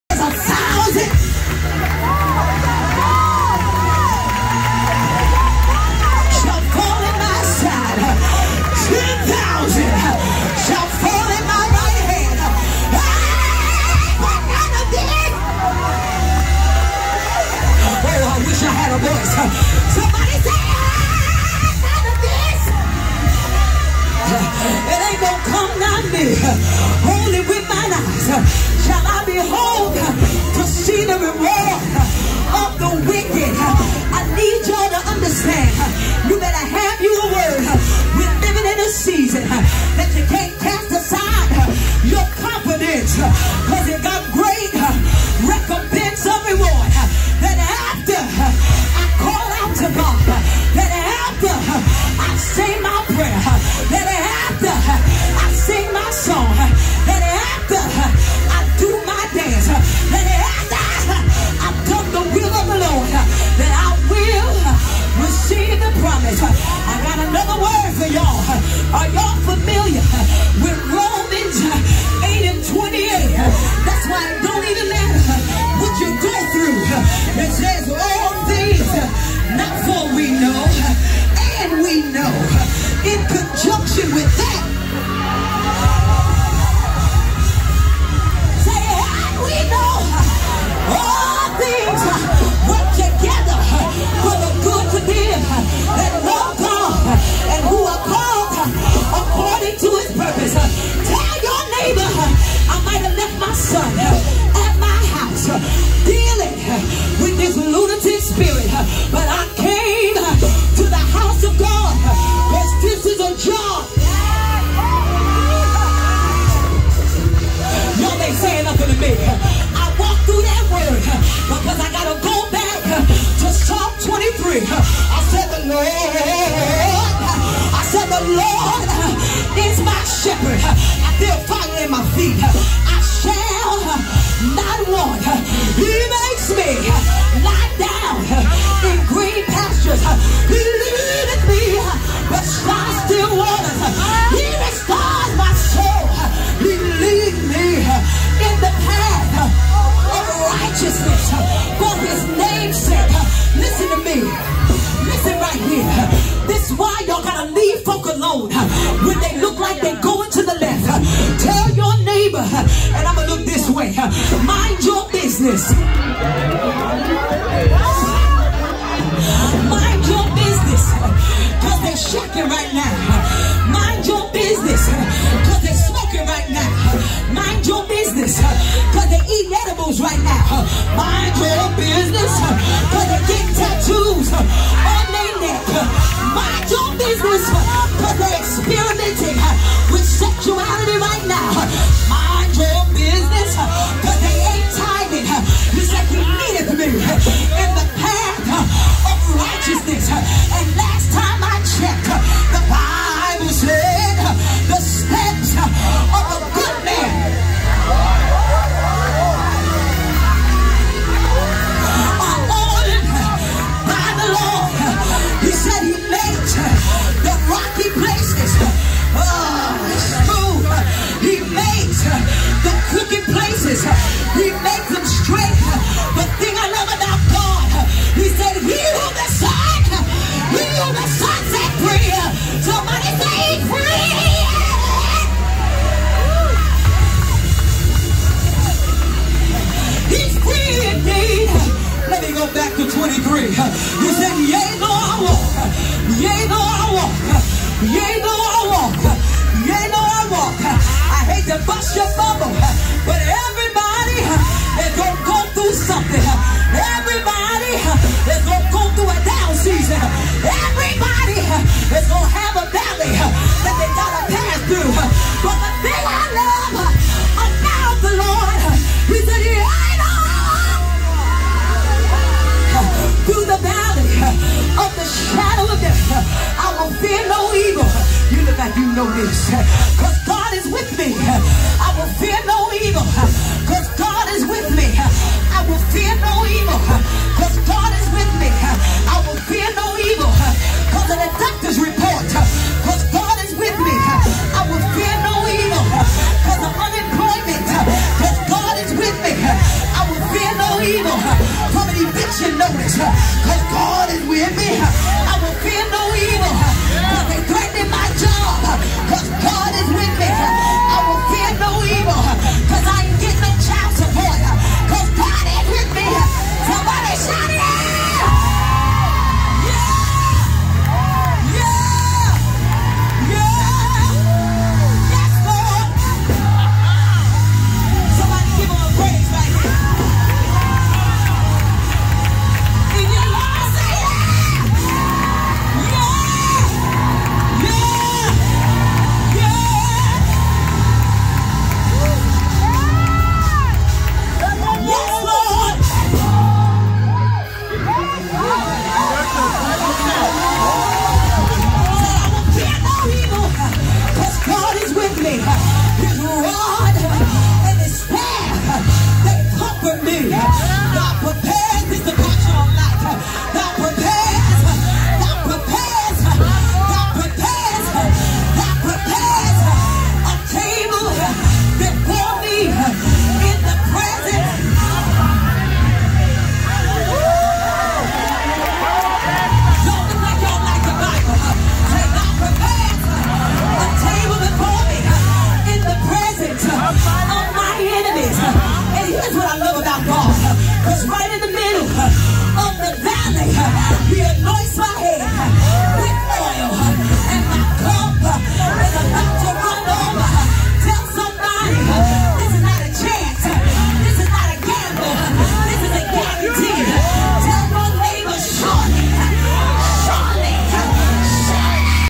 PASTOR FONDREA LEWIS@THE SANCTIFIED CHURCH J.C. THE BELIEVERS GATHERING MISSING FOOTAGE
PASTOR FONDREA  LEWIS WAS GUEST PREACHER  FOR FRIDAY NITE BELIEVERS GATHERING SERVICE  SHE CAME RIGHT ON IN WITH WORD FROM GOD AS THE SPIRIT THE LORD TOOK CONTROL BE BLESSED THANKS FOR WATCHING ❤️ 🙏